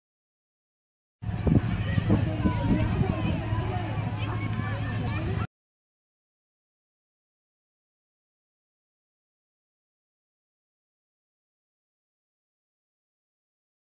Triangulacion de fantasia Copa Pachuca 2014
Triangulacion de fantasia Rafa, Gael, Richi en Nacional Copa Pachuca Mazatlan 2014